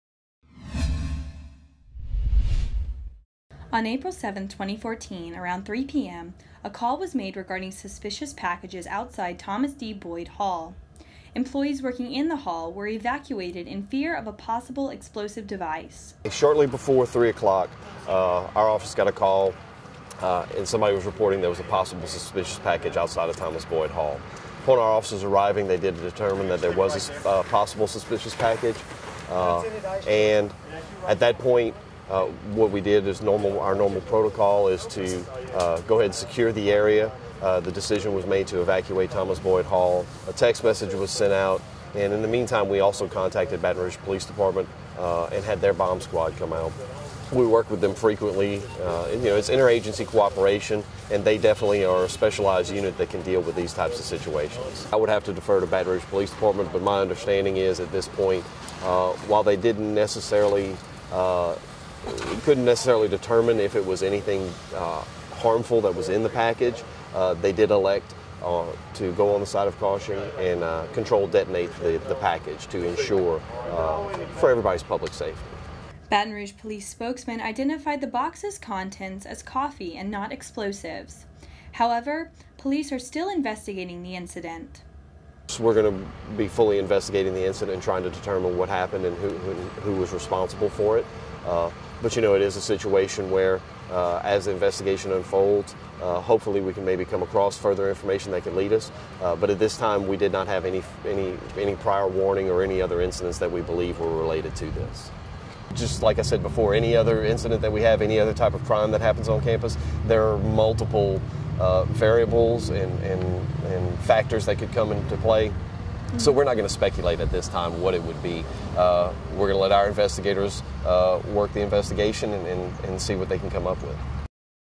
Suspicious Package Detonated on LSU Campus
Video by Staff Videographer Tommy Romanach
The Daily Reveille